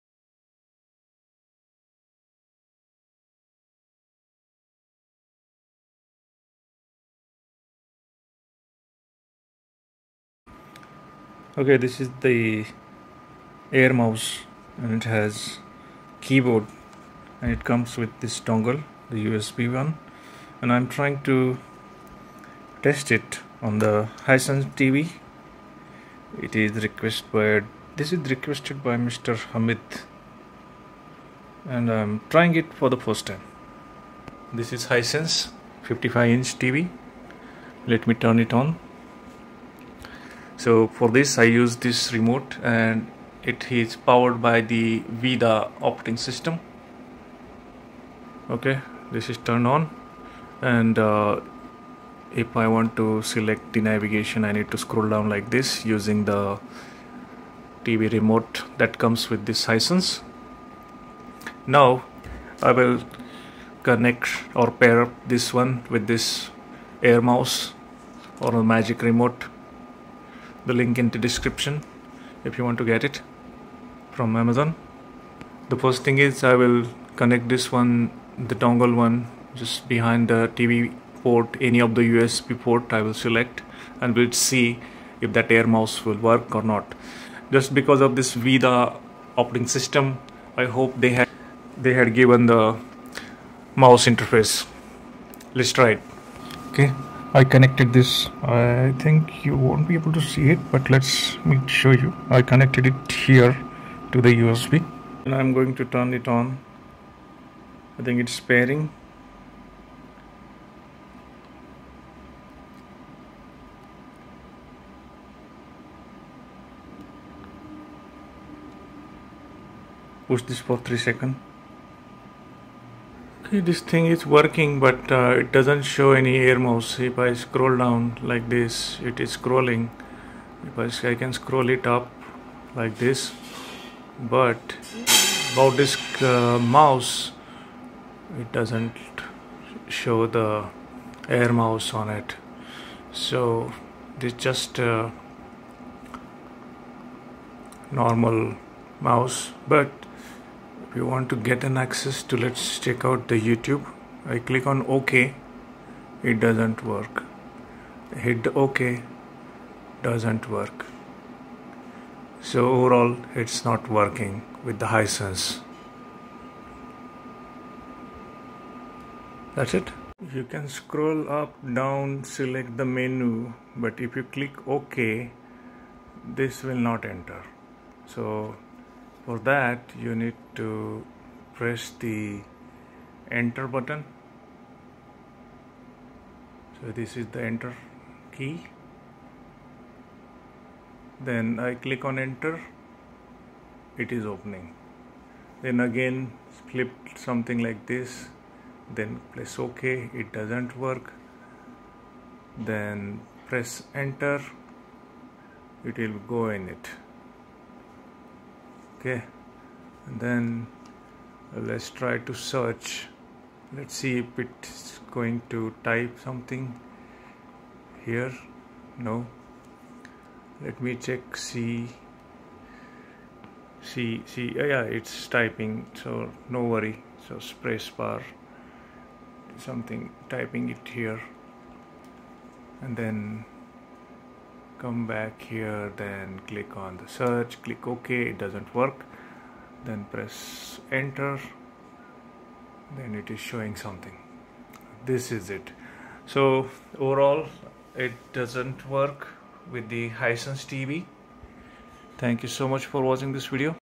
HiSense QLED 4K TV and air mouse with keyboard connection testing || Troubleshooting guide
Title: "HiSense QLED 4K TV & Air Mouse with Keyboard Connection Testing | Troubleshooting Guide"

Description:
📺 In this video, we dive into the world of HiSense QLED 4K TV and Air Mouse with Keyboard connectivity! 🖱️💻 We encountered some initial hiccups, but fear not – we've got the tricks to make it work seamlessly. Join us on this troubleshooting journey and unlock the full potential of your entertainment setup!

🔍 Key Points:
1️⃣ Step-by-step connection guide for HiSense QLED 4K TV and Air Mouse with Keyboard.
2️⃣ Troubleshooting tips for common issues during the setup process.
3️⃣ Uncover hidden features and get the most out of your devices.
4️⃣ Experience a smoother navigation and enhance your TV usage.

🔧 Troubleshooting Tricks:
- Ensure proper pairing procedures.
- Update firmware/software for both devices.
- Adjust TV settings for optimal compatibility.
- Troubleshoot common issues with Air Mouse functionality.

👍 Don't let connectivity challenges get in the way of your ultimate entertainment experience! Watch, learn, and conquer the HiSense QLED 4K TV and Air Mouse setup game with our comprehensive guide. 🚀🔗